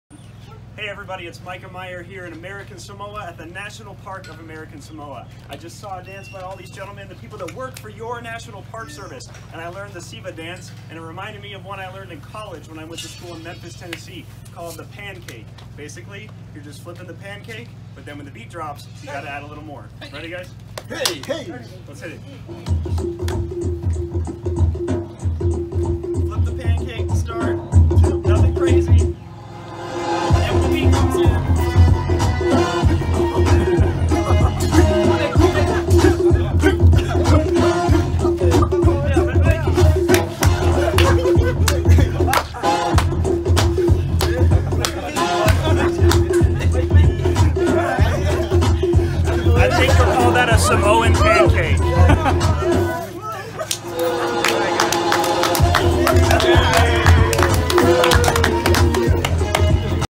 Dancing with National Park of American Samoa employees
Mikah Meyer on his journey to all 417 national parks takes a moment to create the "Samoan Pancake" with some National Park Service employees.